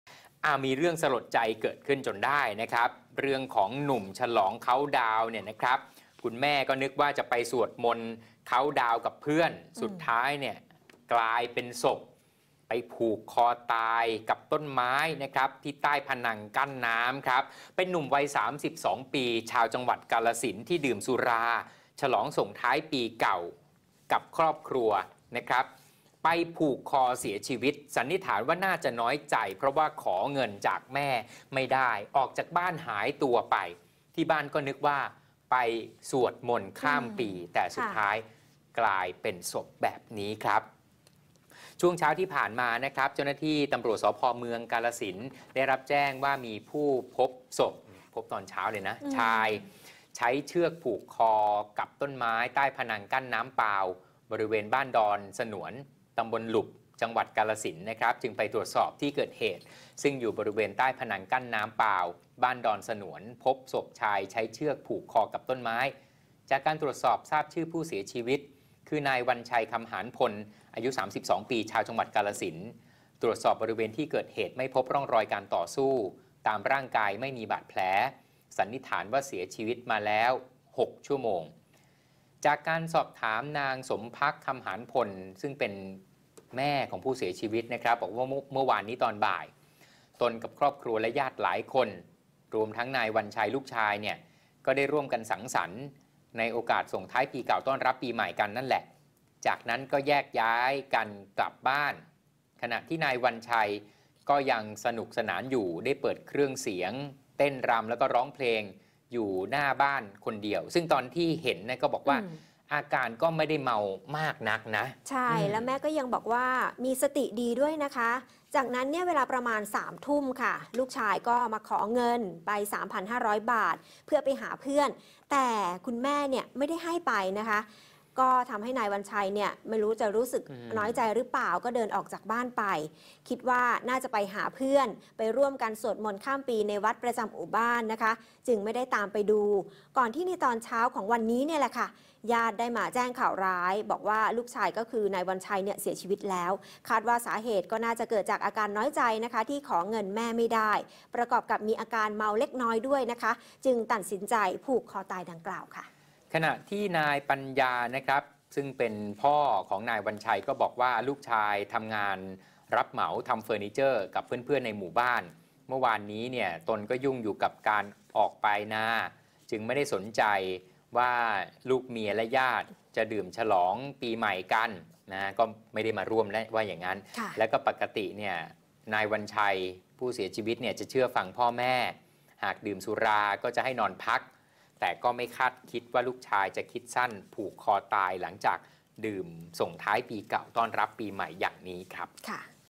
หนุ่มฉลองเคาท์ดาวน์น้อยใจ ขอเงินแม่ไม่ได้ ผูกคอดับ
หนุ่มฉลองเคาท์ดาวน์น้อยใจ ขอเงินแม่ไม่ได้ ผูกคอดับ 01/01/2020